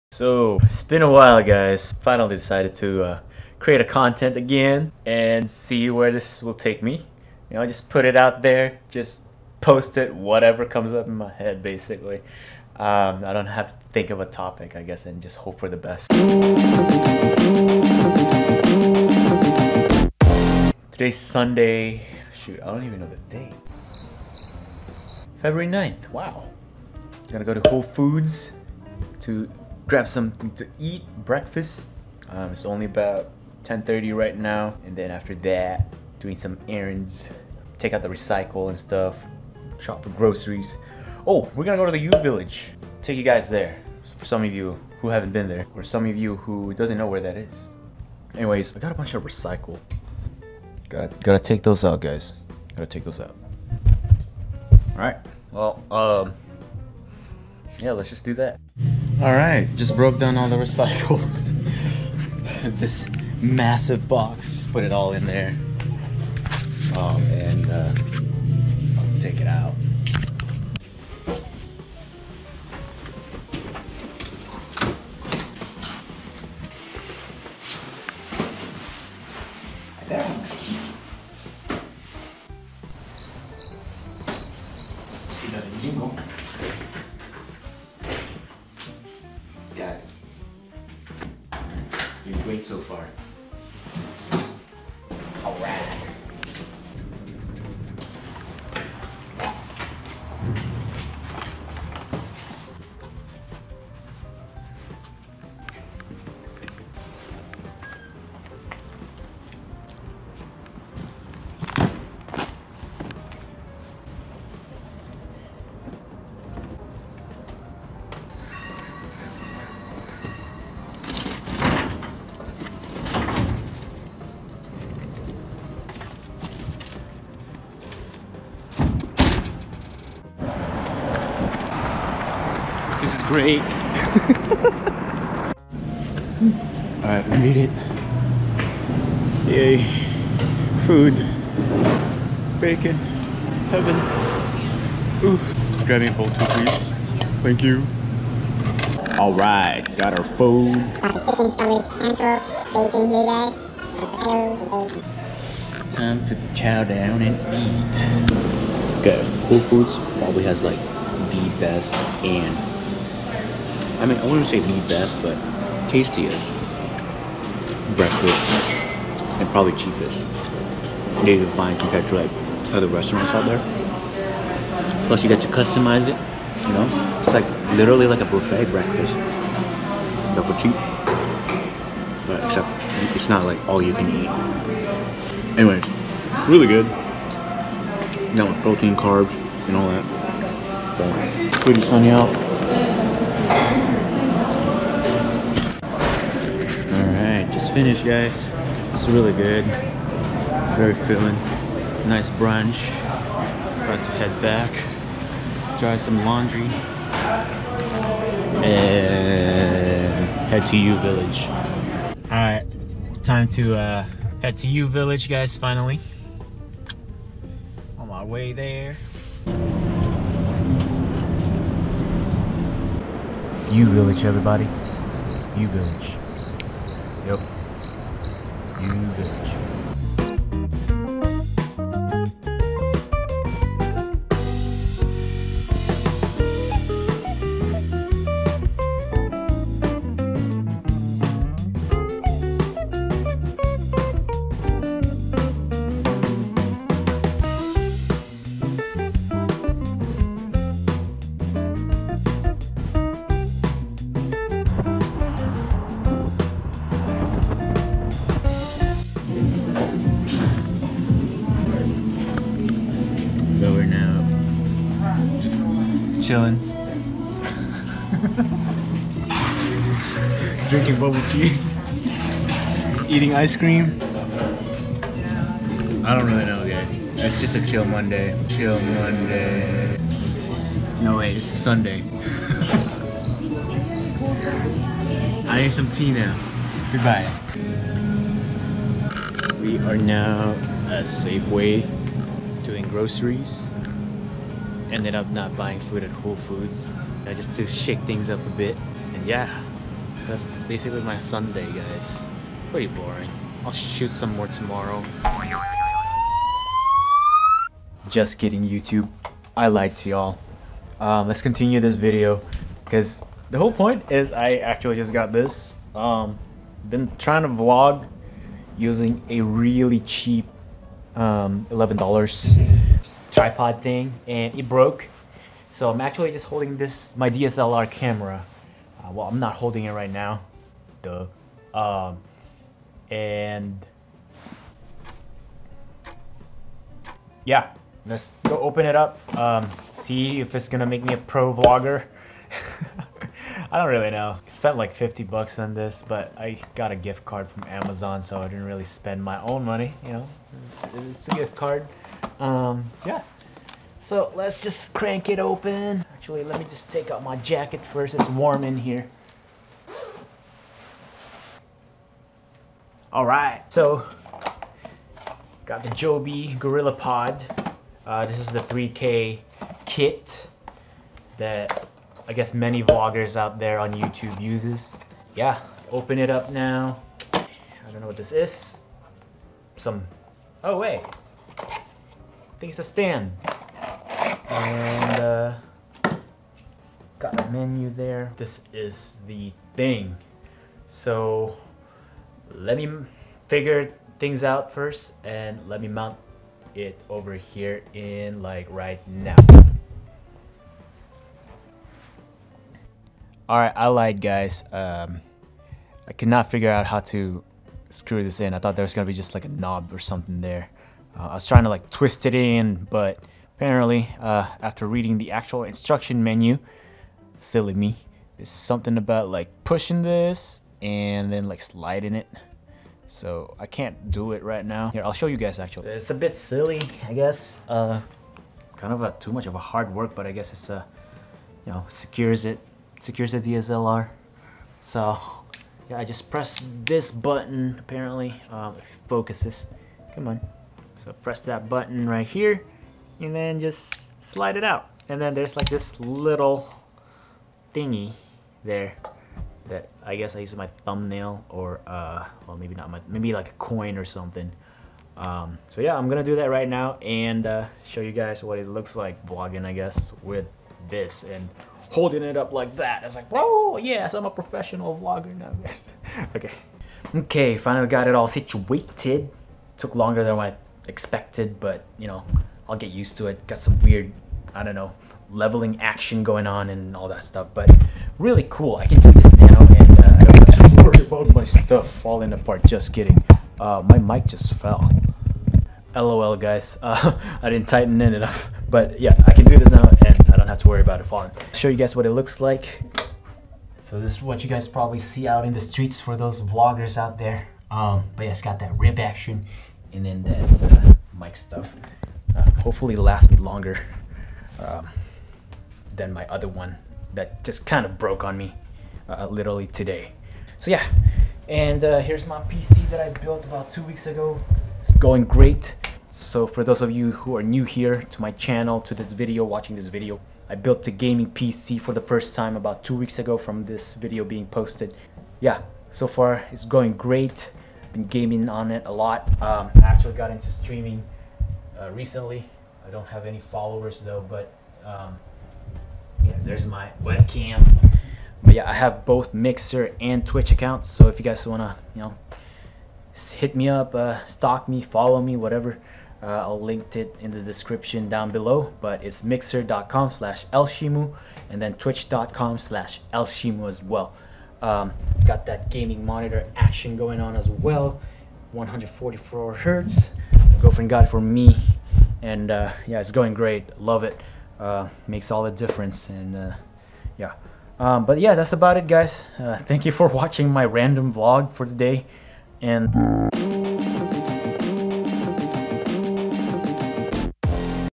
Joby GorillaPod 3K Kit unboxing *Best Vlogging Tripod?* | Weekend Vlog
Unboxing the Joby GorillaPod 3K Kit. Is this the BEST Vlogging Tripod? Also, this my first weekend vlog for this channel!